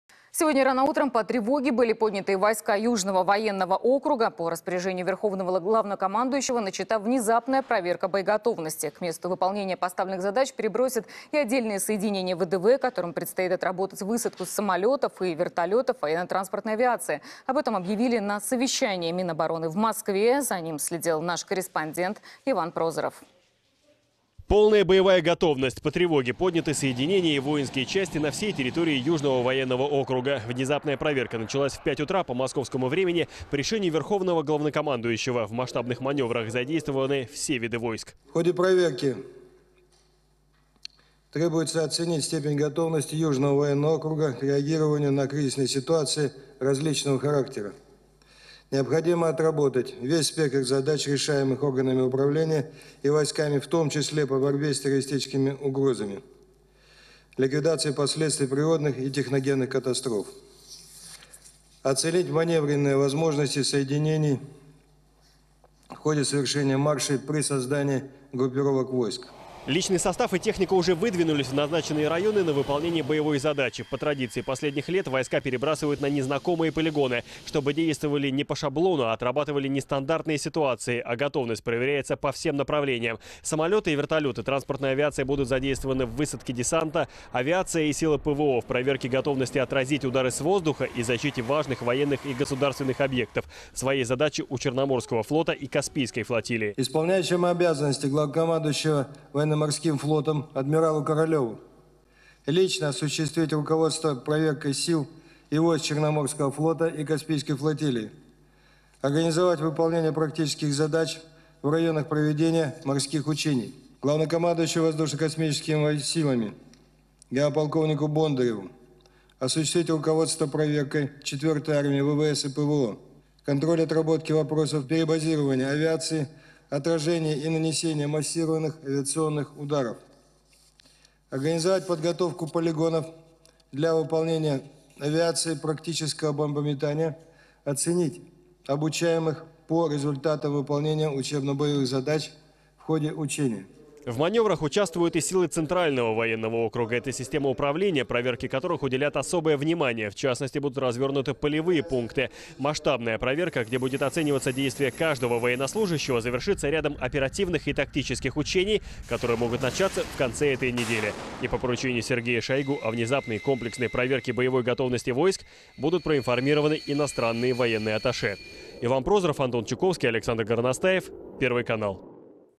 Внезапная проверка боеготовности в Южном военном округе
Сегодня рано утром по тревоге были подняты войска Южного военного округа. По распоряжению Верховного главнокомандующего начата внезапная проверка боеготовности. К месту выполнения поставленных задач перебросят и отдельные соединения ВДВ, которым предстоит отработать высадку с самолётов и вертолётов военно-транспортной авиации. Об этом объявили на совещании Минобороны в Москве.

Полная боевая готовность. По тревоге подняты соединения и воинские части на всей территории Южного военного округа. Внезапная проверка началась в 5 утра по московскому времени по решению Верховного главнокомандующего. В масштабных маневрах задействованы все виды войск.

"В ходе проверки требуется оценить степень готовности Южного военного округа к реагированию на кризисные ситуации различного характера. Необходимо отработать весь спектр задач, решаемых органами управления и войсками, в том числе по борьбе с террористическими угрозами, ликвидации последствий природных и техногенных катастроф. Оценить маневренные возможности соединений в ходе совершения маршей при создании группировок войск", – сообщил Сергей Шойгу.

Личный состав и техника уже выдвинулись в назначенные районы на выполнение боевой задачи. По традиции последних лет, войска перебрасывают на незнакомые полигоны, чтобы действовали не по шаблону, а отрабатывали нестандартные ситуации. А готовность проверяется по всем направлениям. Самолёты и вертолёты транспортной авиации будут задействованы в высадке десанта. Авиация и силы ПВО — в проверке готовности отразить удары с воздуха и защите важных военных и государственных объектов. Свои задачи у Черноморского флота и Каспийской флотилии.

"Исполняющему обязанности главнокомандующего Военно-Морским флотом адмиралу Королеву поручено лично осуществить руководство проверкой сил и войск Черноморского флота и Каспийской флотилии, организовать выполнение практических задач в районах проведения морских учений. Главнокомандующему Воздушно-космическими силами генерал-полковнику Бондареву осуществить руководство проверкой 4-й армии  ВВС и ПВО, контроль отработки вопросов перебазирования авиации, отражения и нанесения массированных авиационных ударов. Подготовить полигоны для выполнения авиацией практического бомбометания, оценить обучаемых по результатам выполнения учебно-боевых задач в ходе учения", – сказал Сергей Шойгу.